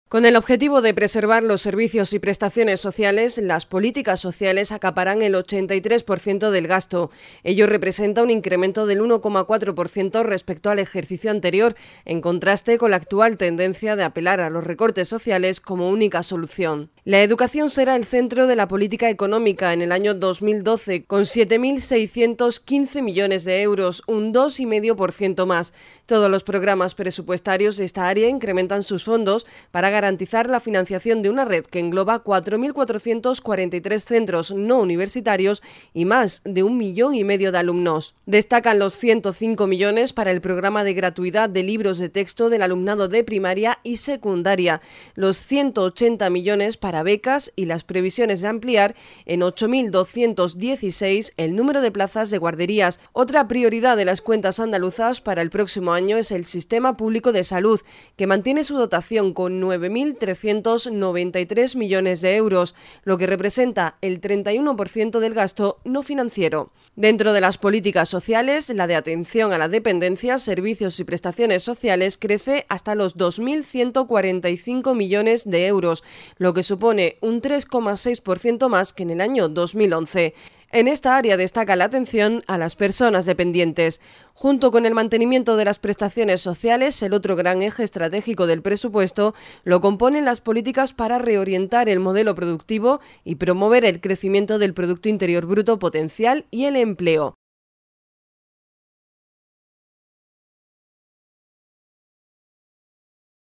Almería Noticias Canal 28 Tv - Educación y empleo, los pilares de los presupuestos de 2012
El proyecto de presupuesto, que será remitido al Parlamento andaluz para su debate y aprobación, cuenta con unos recursos totales de 32.020,1 millones de euros, lo que supone un incremento del 1,1% respecto a 2011. El volumen inicialmente previsto se ha ajustado en 52,6 millones de euros tras la publicación, el sábado 22 de octubre, del Real Decreto 1498/2011, que establece la reversión a la Administración central de las competencias sobre la cuenca del Guadalquivir.